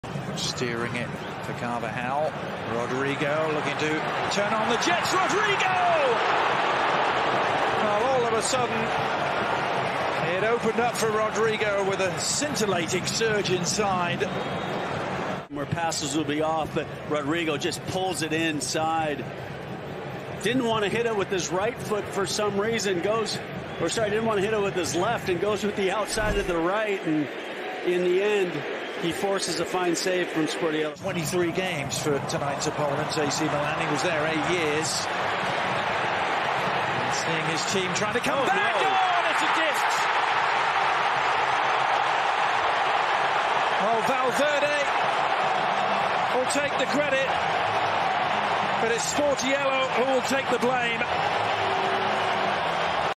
Rodrigos Scintillating Surge Sizzles with Skill and Precision
Marjoriech steering it for Kava Hal Rodrigo looking to turn on the jets Now all of a sudden it opened up for Rodrigo with a scintillating surge inside More passes will be off but Rodrigo just pulls it inside Didn't want to hit him with his right foot for some reason Sorry didn't want to hit him with his left foot and goes with the outside of the right In the end he forces a fine save from Sportiello 23 games for tonight's opponent AC Milan He was there eight years Seeing his team trying to come back on as he gets Valverde will take the credit but it's Sportiello who will take the blame